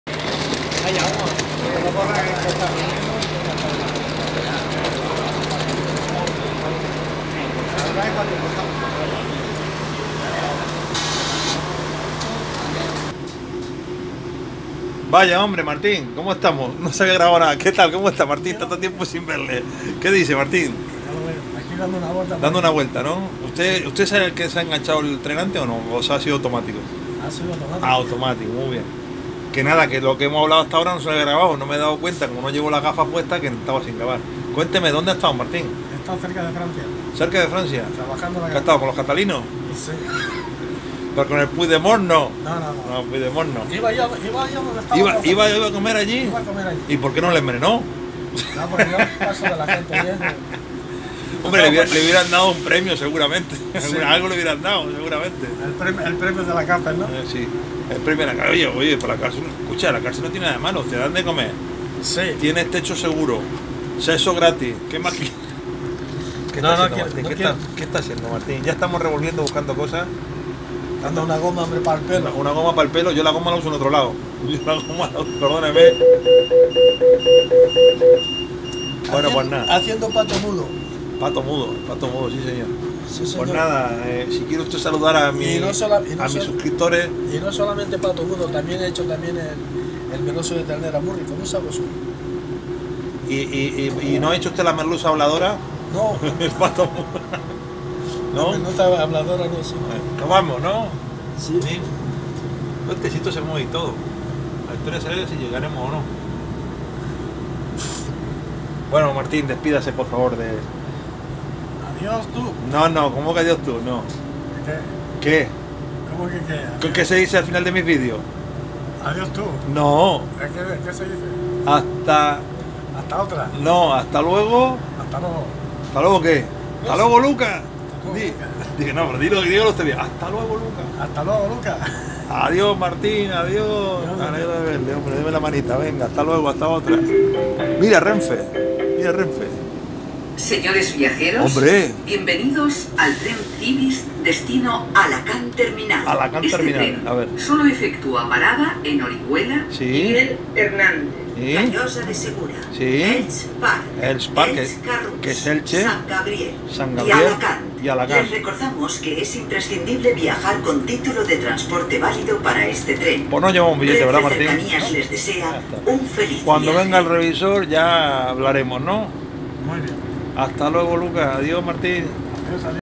El retorno de Martin y Puigdemont!!!
Martin nos viene a hablar sobre los perros flautas y gatos violines, Martin gracias por tu amistad!!